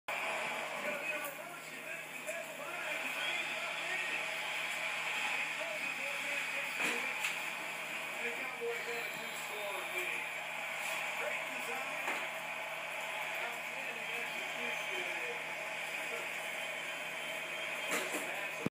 Dez Bryant Touchdown Pass to Jason Witten ( NFL Trick Plays )
Dallas Cowboys vs Detroit Lions